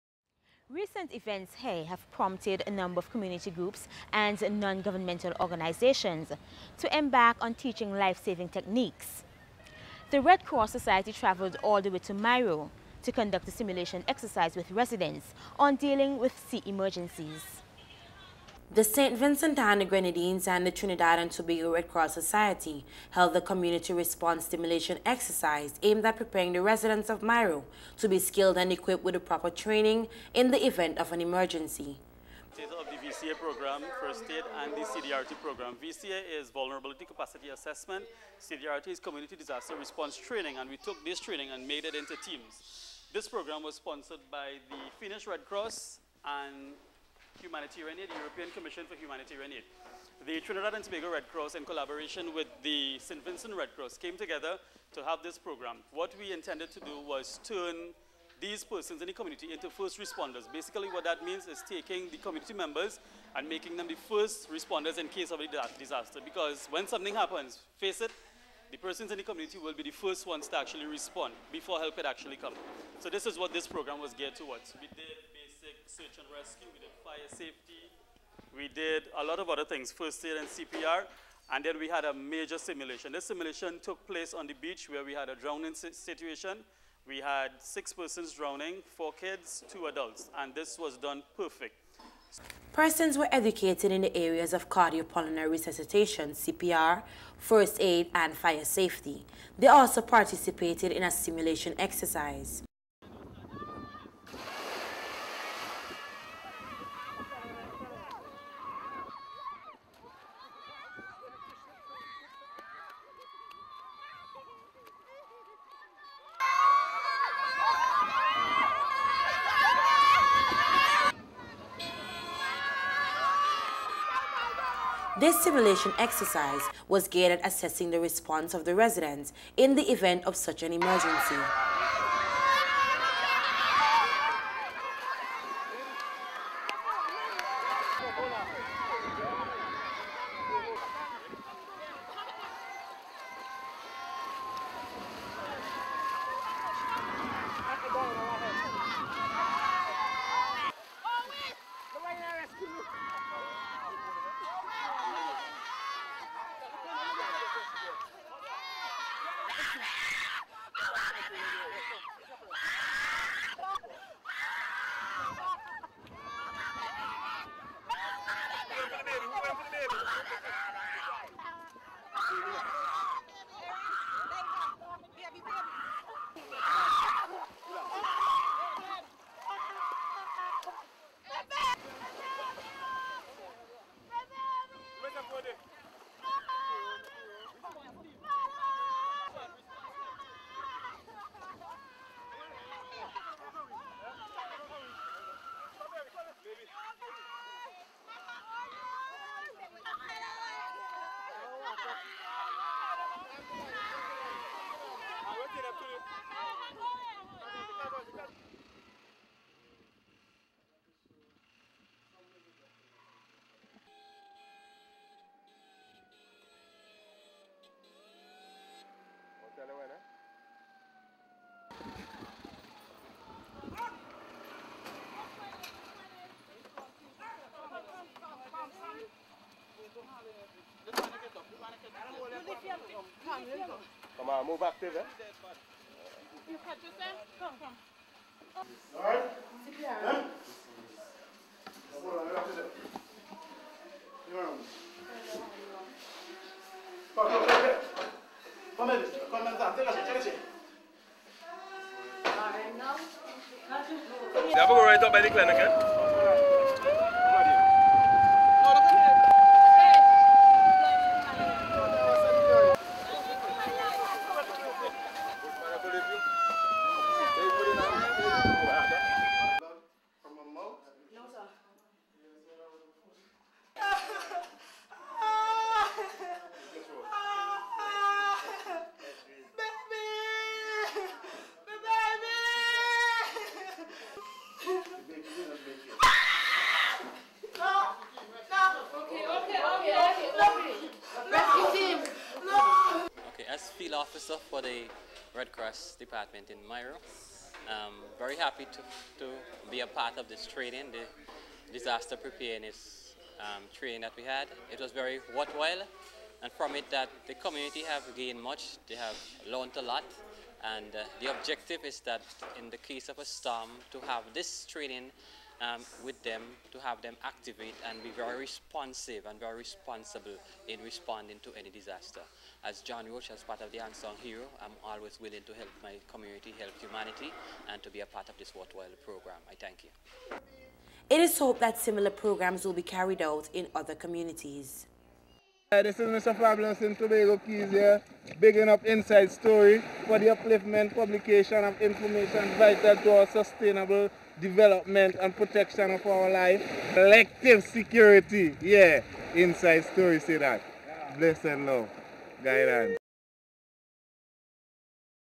Mayreau Simulation Exercise
This simulation exercise was completed in Mayreau as part of the St. Vincent and the Grenadines Red Cross Community Disaster Preparedness Programme. The programme is funded by the European Union Humanitarian Aid Department and the Finnish Red Cross.